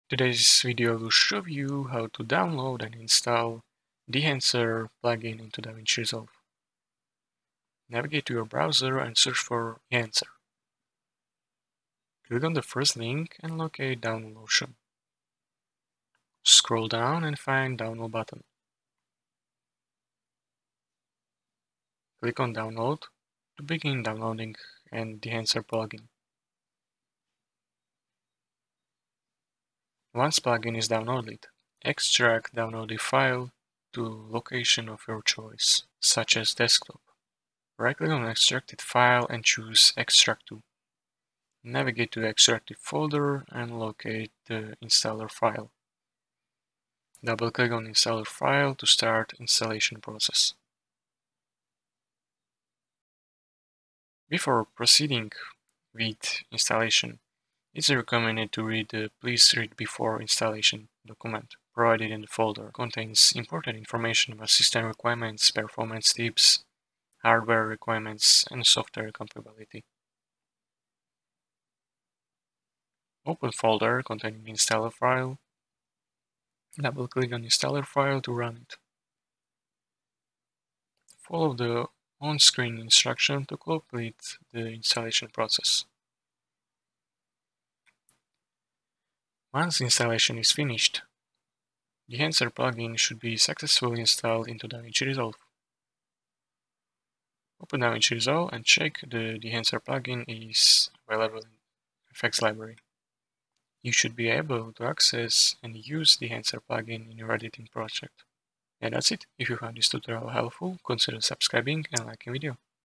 Davinci Resolve : How to Install Dehancer (2025)
Summarize:

1. **Download Dehancer :**
   - Navigate to your browser and search for "Dehancer Davinci Resolve."
   - Click on the first link and locate the download option.
   - Scroll down and find the download button.
   - Click on "Download" to begin downloading the Dehancer plugin.

2. **Extract and Install:**
   - Once the plugin is downloaded, extract the downloaded file to a location of your choice, such as the desktop.
   - Right-click on the extracted file and choose "Extract to [filename]."
   - Navigate to the extracted folder and locate the installer file.
   - Double-click on the installer file to start the installation process.

3. **Read Installation Guide:**
   - Before proceeding with the installation, it's recommended to read the "Please Read Before Installation" document provided in the folder.
   - This document contains important information about system requirements, performance tips, hardware requirements, and software compatibility.

4. **Run Installer:**
   - Open the folder containing the installer file.
   - Double-click on the installer file to run it.
   - Follow the on-screen instructions to complete the installation process.

5. **Installation Complete:**
   - Once the installation is finished, the Dehancer plugin should be successfully installed into DaVinci Resolve 18.

6. **Verify Installation:**
   - Open DaVinci Resolve 18 and check if the Dehancer plugin is available in the effects library.
   - You should be able to access and use the Dehancer plugin in your editing projects.

By following these steps, you can easily install the Dehancer plugin into DaVinci Resolve 18 and enhance your editing capabilities.